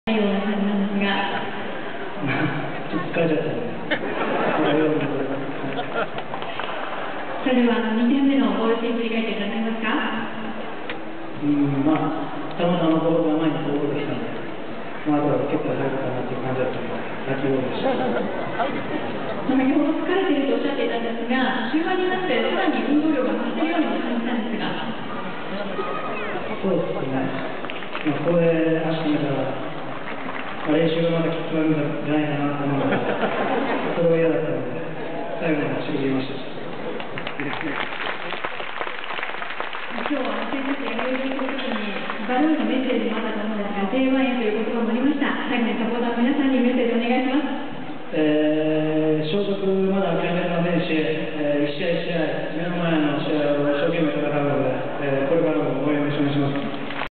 12.09.02水戸vs鳥取、西岡謙太選手インタビュー
試合後、水戸・西岡謙太選手インタビューです。